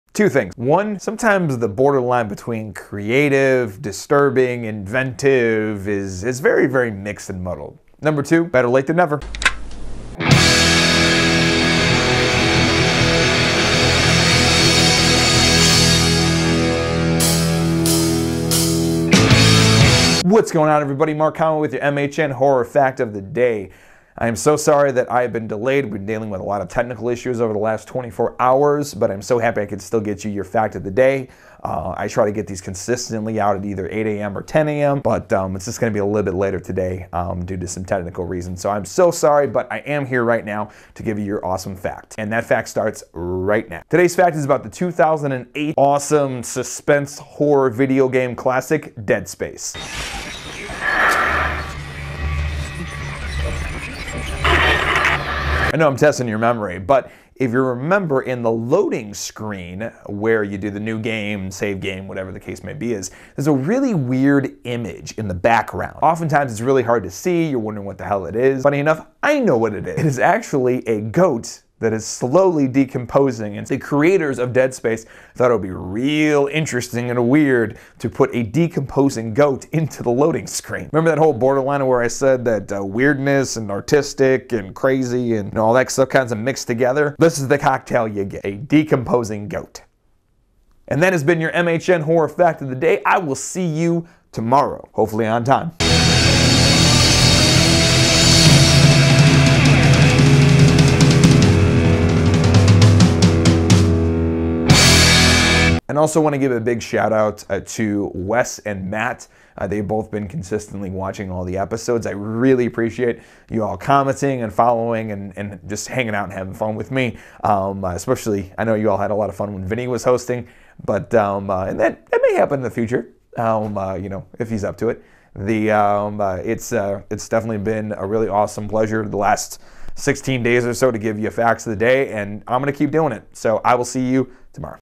MHN Horror Fact Of The Day 18- Dead Space
Today's fact of the about the 2008 awesome horror video game dead space.  More specifically about loading screen.  I was not able to get the loading screen in the video.

Slasher: @midwesthorrornetwork